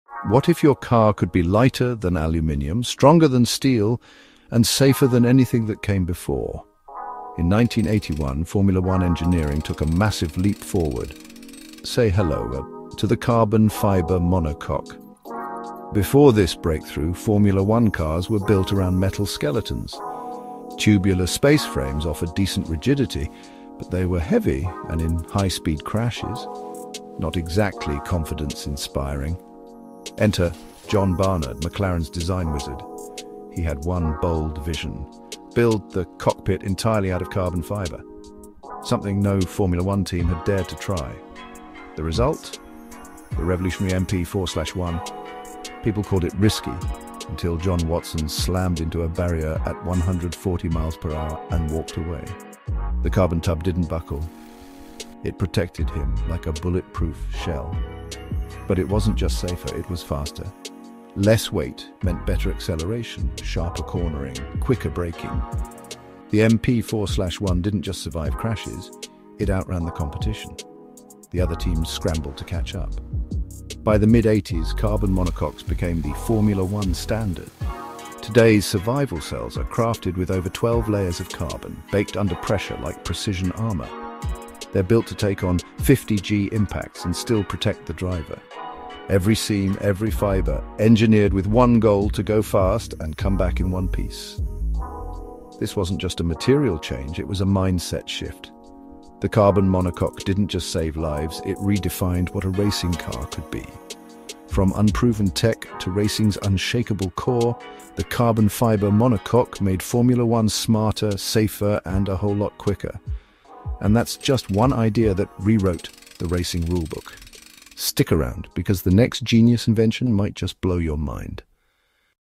Genius Ideas That Changed Racing Forever – The Carbon Fibre Monocoque
What if your car could be lighter than aluminium, stronger than steel, and safer than ever before? Discover how the revolutionary carbon fibre monocoque transformed Formula 1 racing in our latest video! 🏎️💨 Join us as we explore the ground-breaking innovations of the 1981 McLaren MP4/1, designed by John Barnard, and learn how this game-changing technology not only enhanced safety but also catapulted performance. From jaw-dropping crash tests to the rise of carbon composites in modern racing, we unpack the evolution of F1 engineering. Buckle up, and prepare for a thrilling ride through motorsport history!

OUTLINE:

00:00:00 Genius Ideas That Changed Racing Forever – The Carbon Fibre Monocoque